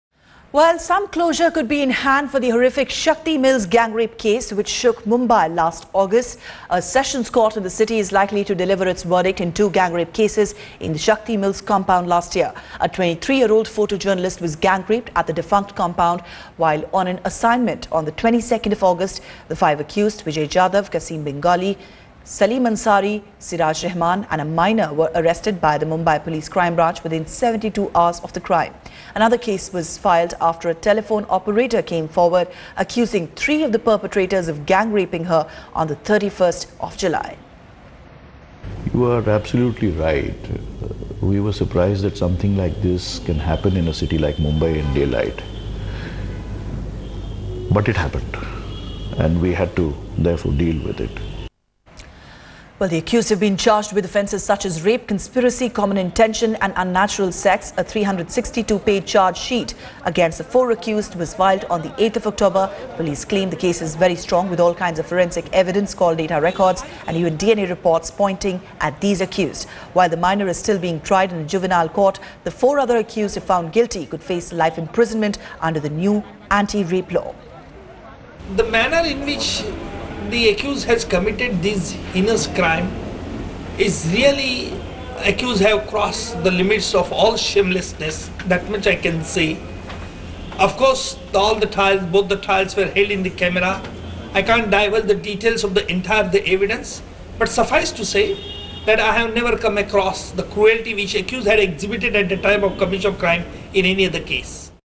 Shakti Mills gang-rape case verdict expected today
A sessions court in Mumbai is likely to deliver its verdict in the Shakti Mills gang-rape case that happened to a photojournalist in August 2013. 5 men were arrested and charged for the crime. Another rape survivor also accused the same five men -- one of whom is a minor. The 4 adult men will be either convicted or acquitted today if the verdict comes in.